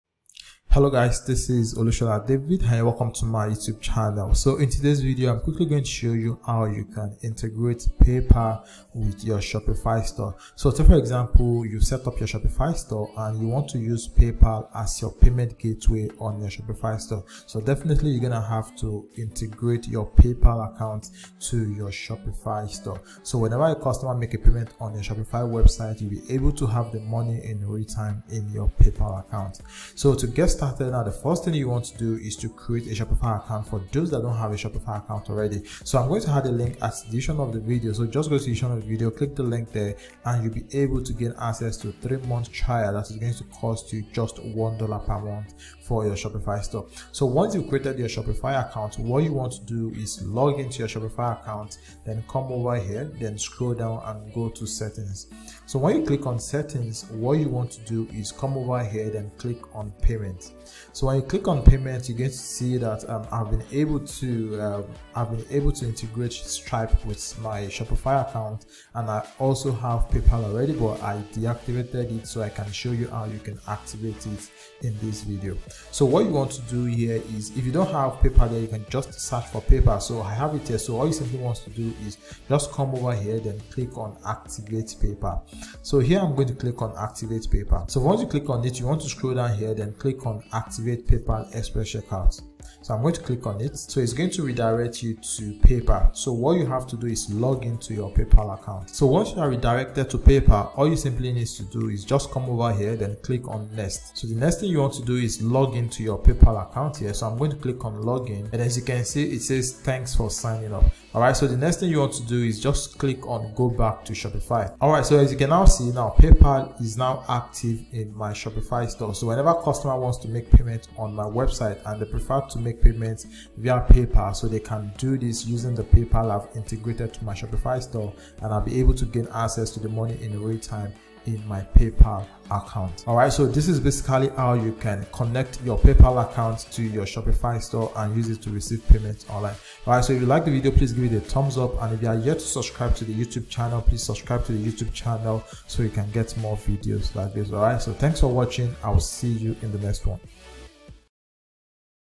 How To Integrate PayPal Payments Into Shopify [Step By Step]
Discover how you can add PayPal payments to your Shopify and receive PayPal payments from your customers.